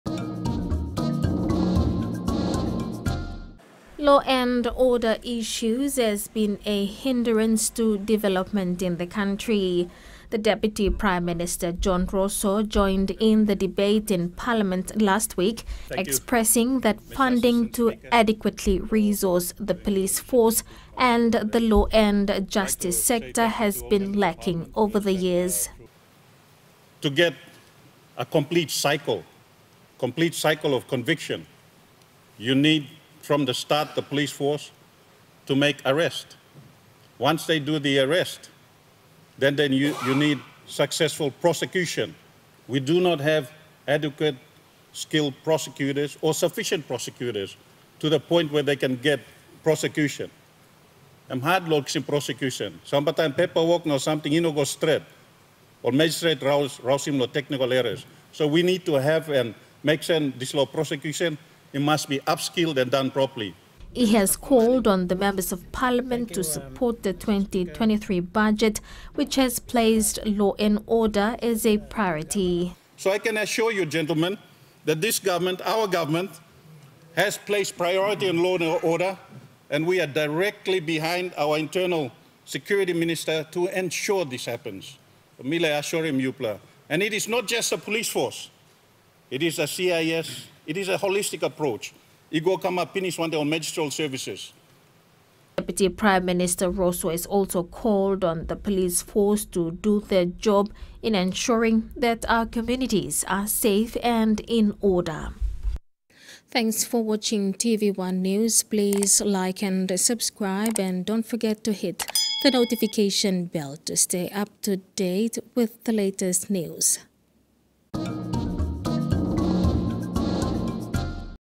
Law & Order Budget
Deputy Prime Minister John Rosso said the Government has placed law and order as a priority in the 2023 National Budget.

He said adequate funding will be provided to the Police force including the Law and Justice sector.

Rosso made these remarks during a recent parliament debate.
In today’s budget presentation, an increase of K401 million was revealed for the sector.